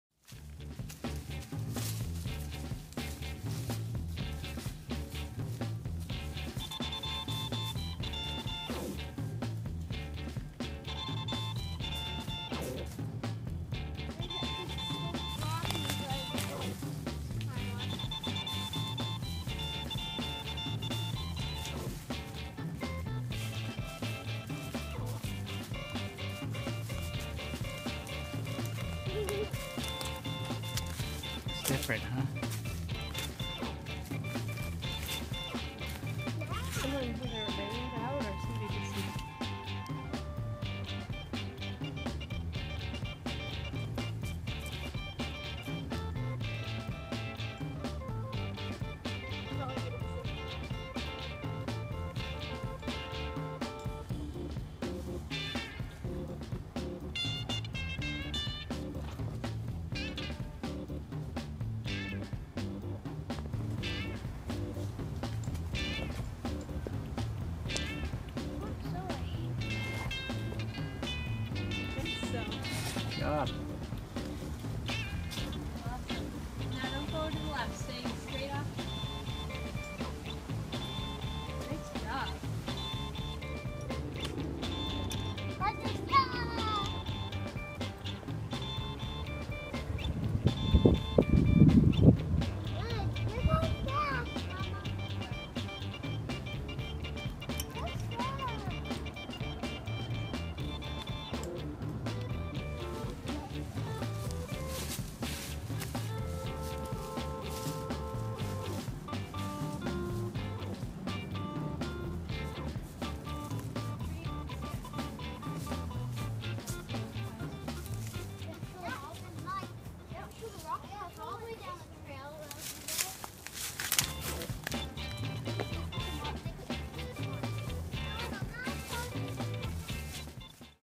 Rock climbing with kids - Mount Forist | White Mountains, New Hampshire
A little after school cragging at Mount Forist in Berlin, New Hampshire